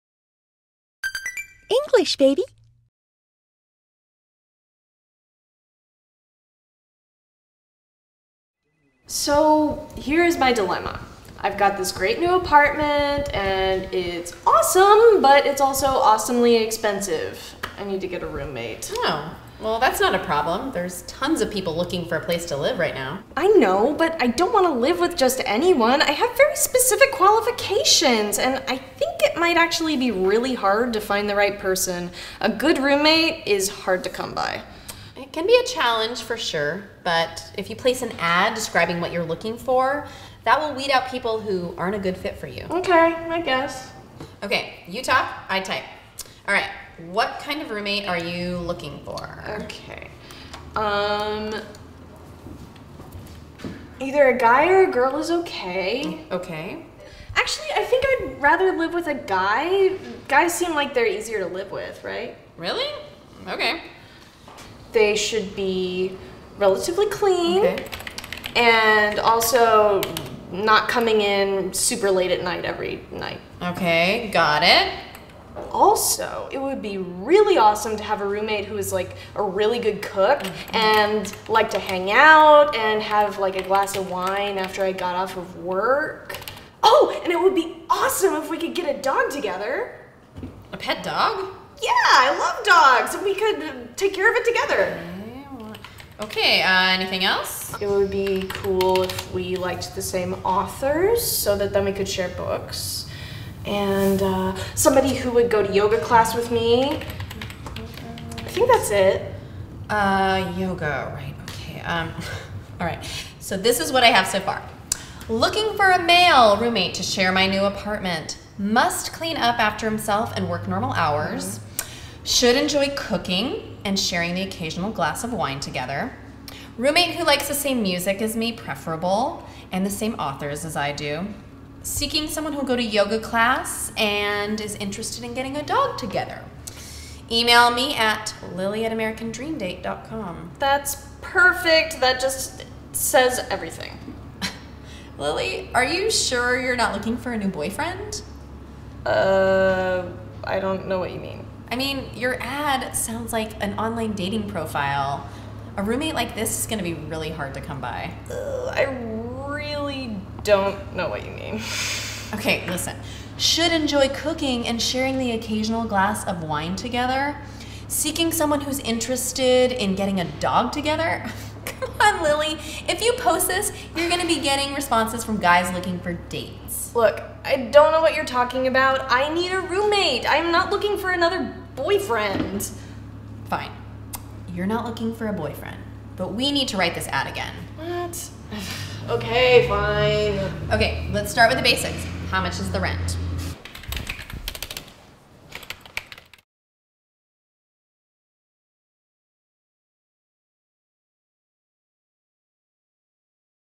Learn English with this roommates English lesson Hard to Come By
Credit :  Englishbaby.com website
When you were a kid, you probably spent time looking for things you thought were valuable. Perhaps you looked for money on the ground, searched for shells at the beach, or went digging for diamonds in the earth. Even though they were difficult to find, you tried your hardest. You knew that if something is hard to come by, you should spend your time looking for it.
Dilemma n.

Definition :  difficult decision or problem
Example : Trying to decide what to wear is a serious dilemma for some teenagers.
Hard to come by expr.

Definition :  difficult to find
Example : Perfect shoes can be hard to come by, so sometimes you have to shop for a while.
Occasional adj.

Definition : Once in a while; from time to time
Example :  I drink an occasional beer after work.
Good fit n.

Definition : good match, good combination
Example :  We just don’t feel that you’re a very good fit with this company.